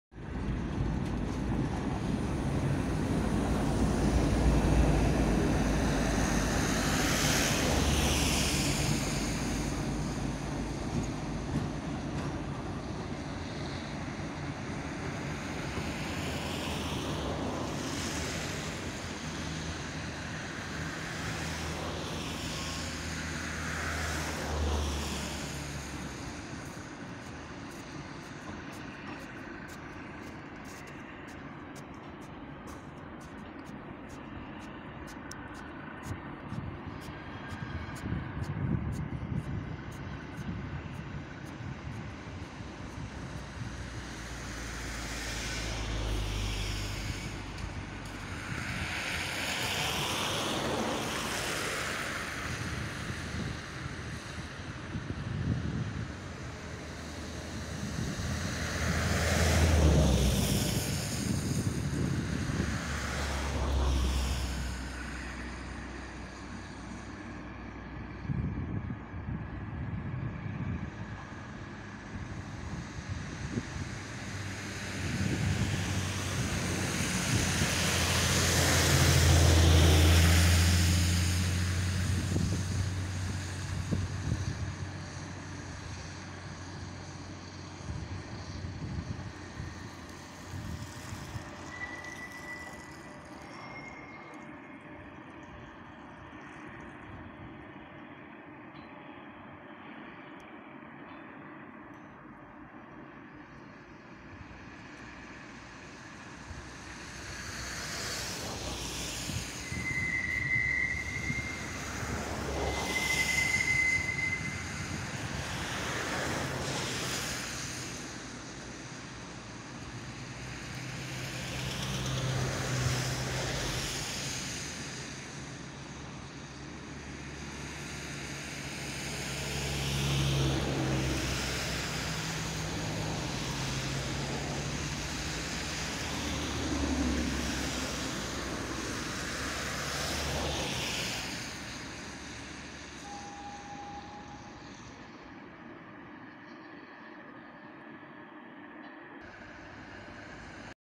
"Надвиг на горку и звуки вагонных замедлителей" Чмэ3-4259 ст.Харьков сортировочный Северный Пост
VID_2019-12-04_15:41:47 чмэ3 на горку Харьков сортировочный.mp4